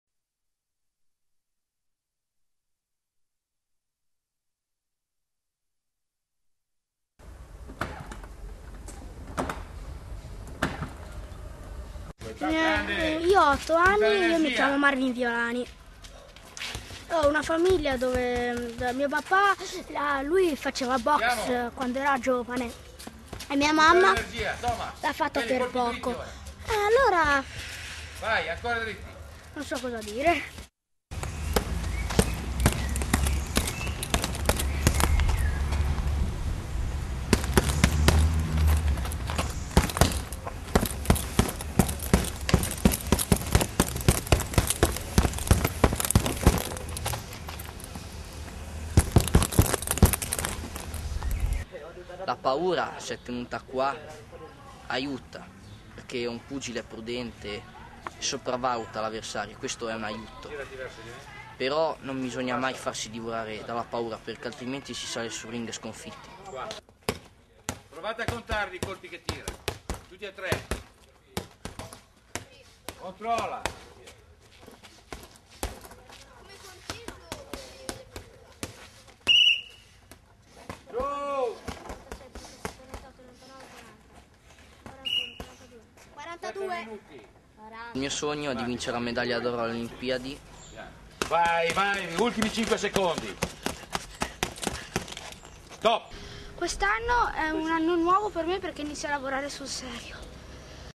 ROBERTO E I GIOVANI BOXEURS DEL FAVARO
E la storia di un gruppo di giovani di diverse età allenati da Roberto Carli, che svolge per amore dello sport e per l'insegnamento questo lavoro in modo assolutamente gratuito. Il gruppo si è creato in uno dei quartieri più disagiati di La Spezia e si allena presso un circolo ricreativo. L'obiettivo dell' allenatore è però quello di creare un centro educativo, realizzando un progetto sportivo di animazione del territorio, aggregante per i giovani. L' ambiente dove Roberto espleta la sua attività istruttiva è piuttosto degradato. La struttura dove i ragazzi si allenano è composta da una stanza per le stagioni fredde ed uno spazio esterno provvisto di una struttura piuttosto fatiscente che supporta i sacconi da allenamento. La convivenza tra i ragazzi di Roberto e gli anziani che trascorrono i pomeriggi nella struttura non sempre è facile vista la conflittualità che si viene a creare tra l'esuberanza giovanile e la suscettibilità senile. Negli ultimi mesi alcuni ragazzini di origine domenicana ed albanese provenienti da un altro quartiere popolare della città vengono accolti con grande ospitalità e cordialità. Lo scopo del filmato è quello di essere uno strumento di informazione sulle tematiche sociali e sullo sport per fornire una visione che induca un cambiamento culturale sulla boxe come sport che obbliga a relazionarsi con l'altro, con la paura, con il coraggio e con i propri limiti all'interno di regole ben precise.